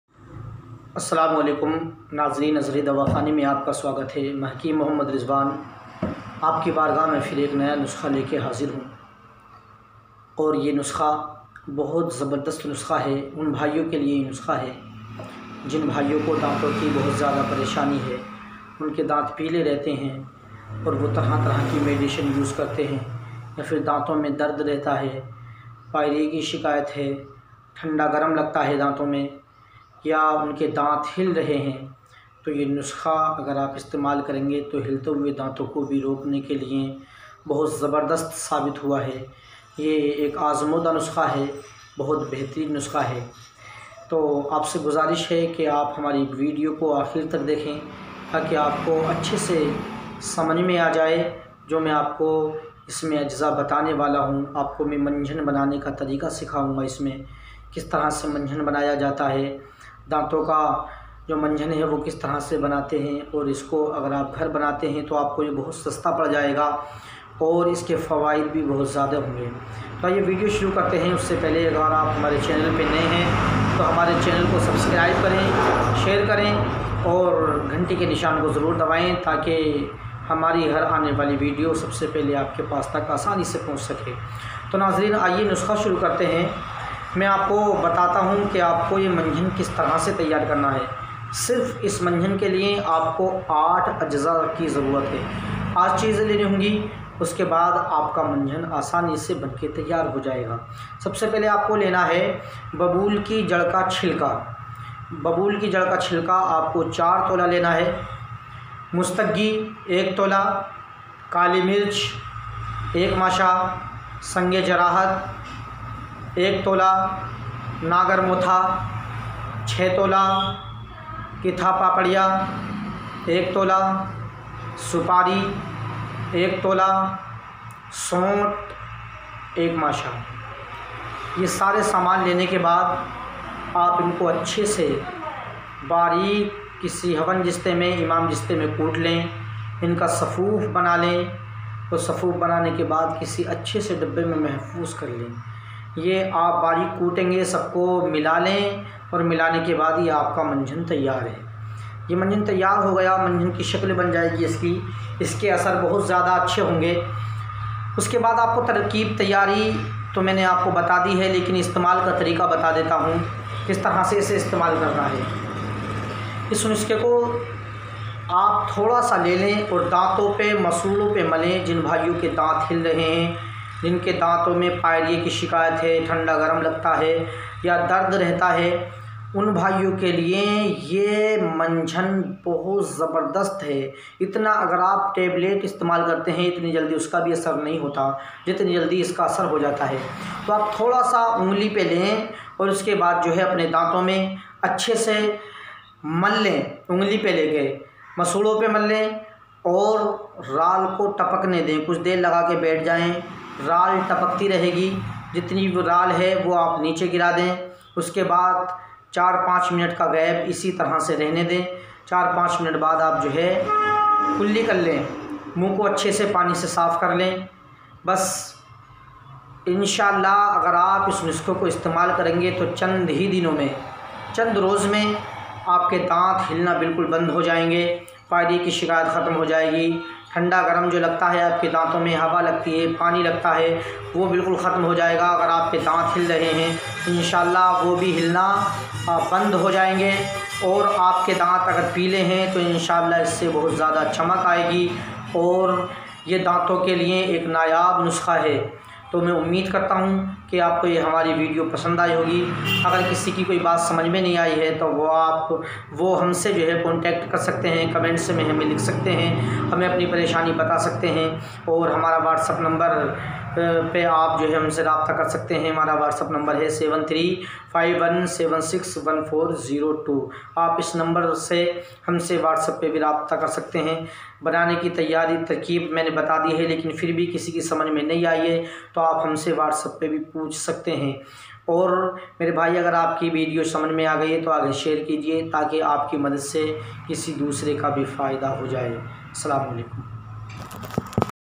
दांत नहीं होंगे 80 साल तक भी खराब यह नुस्खा इस्तेमाल करो
दांतों का दर्द दांतों का हिलना मसूड़ों का फूलना पानी का लगना  दांतों  में बदबू आना पारिया यह वाला नुस्खा इस्तेमाल करें